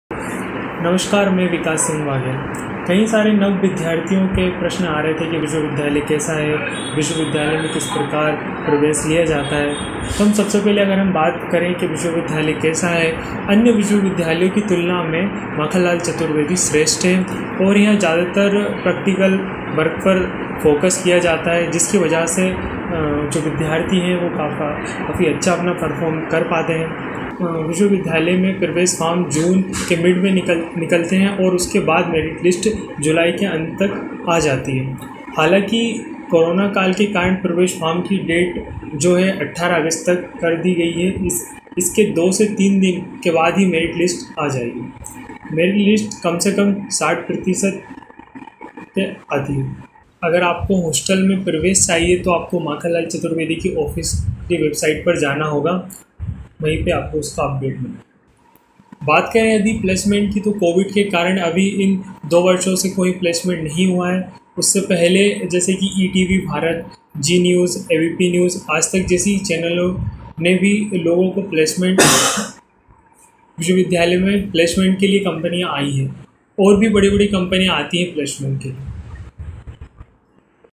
MCU Student Vikas Singh Baghel | All About Information MCU Admission, Course, Fees, and Placement
MCU में प्रवेश को लेकर सारी जानकारी... अध्यनरत छात्र विकास सिंह बघेल से जानें...

कोविड के कारण समूचे भारत की शिक्षा बर्बाद हो रही है... जिसका मुख्य कारण ऑफलाइन कक्षाओं नहीं चलाना उसी को लेकर पत्रकारिता कोर्स में भी काफी दिक्कत हो रहा जैसे प्रैक्टिकल का ना होना... जो सभी प्रोफेशनल कोर्सेज में बहुत आवश्यक है।

अगर आपके मन में और भी सवाल है तो कॉमेंट बॉक्स में पूछे हम उसका उत्तर देने का पूरा प्रयास करेंगे।

धन्यवाद...